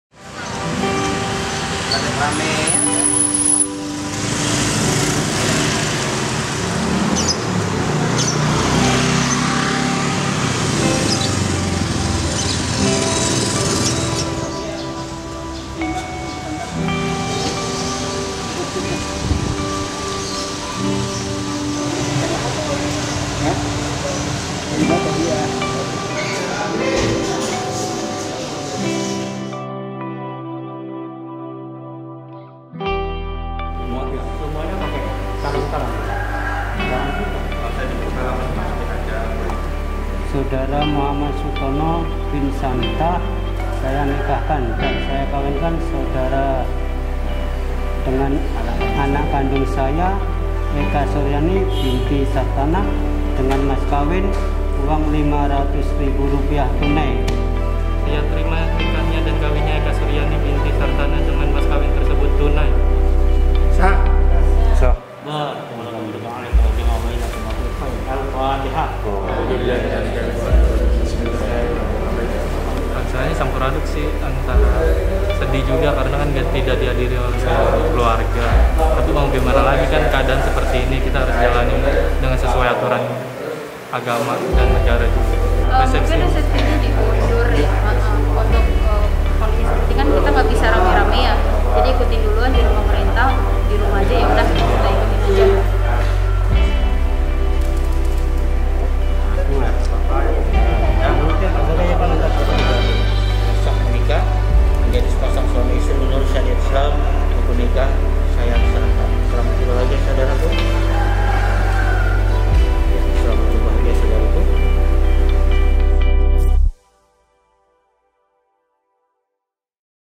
Pelayanan Akad Nikah di KUA Selama Pandemi Covid-19
PASANGAN pengantin Muhamad Sutono dan Eka Suryani melangsungkan prosesi akad nikah di Kantor Urusan Agama Kecamatan Kemayoran, Jakarta, Minggu (12/4/2020).

Selama penerapan Pembatasan Sosial Berskala Besar (PSBB) hanya memperbolehkan akad nikah di Kantor Urusan Agama dan melarang adanya resepsi pernikahan.

Kementerian Agama meminta masyarakat menunda atau menjadwal ulang rencana pelaksanaan pernikahannya (akad nikah) selama darurat covid-19.

Sebab, Kantor Urusan Agama (KUA) menutup pelayanan prosesi akad nikah per awal bulan April.

Adapun pelaksanaan akad nikah hanya dilayani bagi calon pengantin yang sudah mendaftarkan diri sebelum 1 April 2020.

Pelayanan akad tersebut hanya akan dilaksanakan di KUA, bukan di luar KUA.

Kendati demikian, pendaftaran layanan pencatatan nikah tetap dibuka meski dalam keadaan wabah covid-19.

Namun, mekanisme pendaftaran tidak dilakukan dengan tatap muka di KUA, tetapi secara daring melalui laman simkah.kemenag.go.id.

Pelaksanaan akad nikah juga tidak akan dilakukan di masa darurat covid-19.

Perkembangan terkait waktu pelaksanan akad yang diizinkan akan terus diperbaharui.